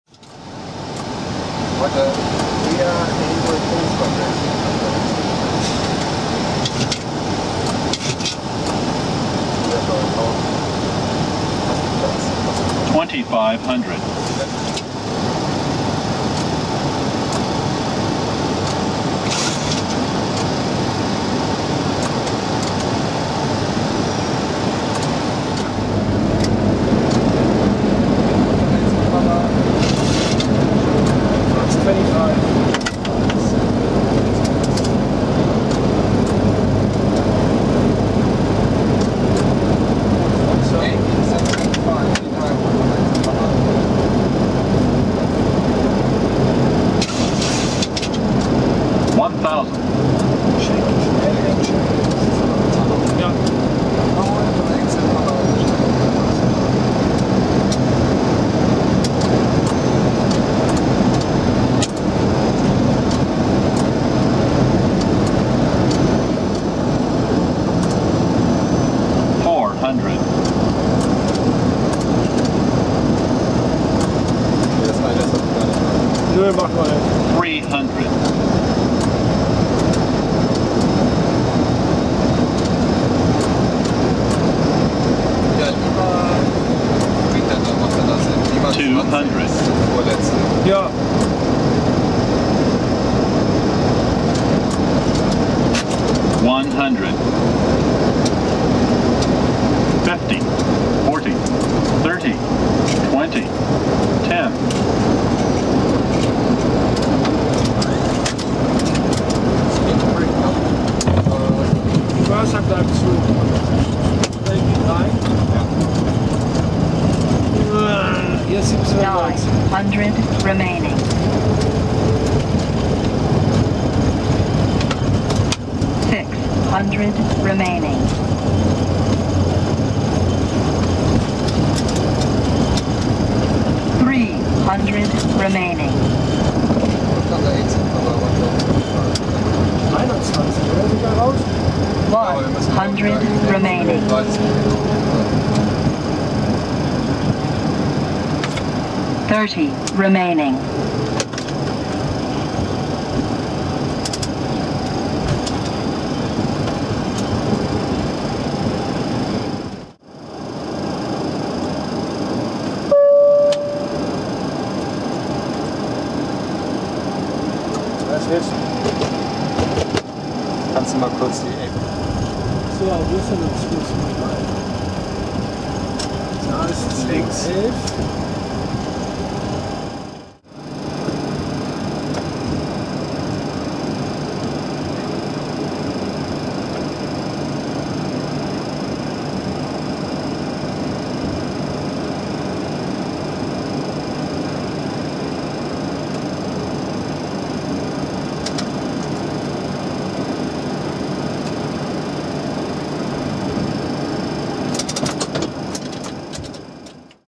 Very Unusual but Awesome Landing in Frankfurt - Cockpit View!
While approaching Frankfurt's 25L, our captain had the idea to perform a swing to 25C, because the parking position was at the end of the airport, and so we could roll all the way down the runway and save a lot of time.
The tower gave the permission to do so, and we landed without reversers and speedbrakes. I sat on the jumpseat and captured it all for you guys ;-)
I hope you enjoy this video nearly as much as I enjoyed the landing!

the plane was a 737-300.